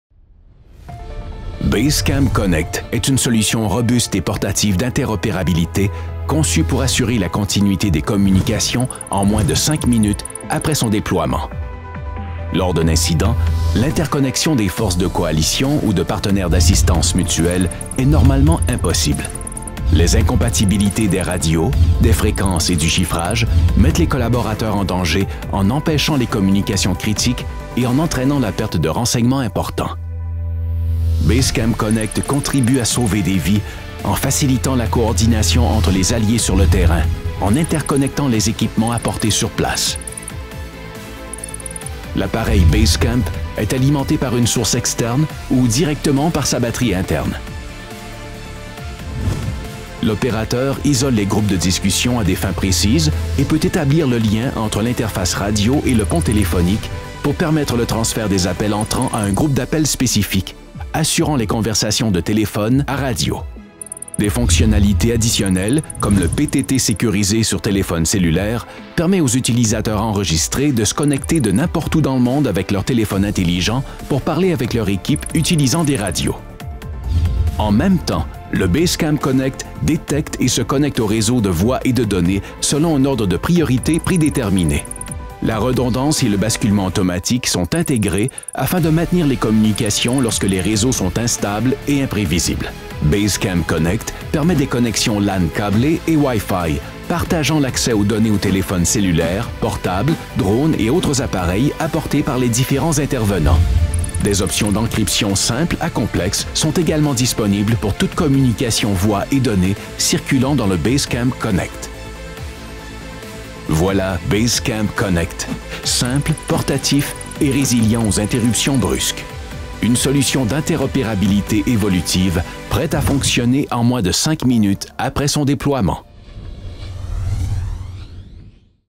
Solutions de communications déployables - Base Camp Connect
Si vous ne pouvez pas vous permettre de défaillances dans vos communications, vous pouvez compter sur votre Base Camp Connect, quelle que soit la distance, vos partenaires ou si l’infrastructure est surchargée, hors service ou indisponible.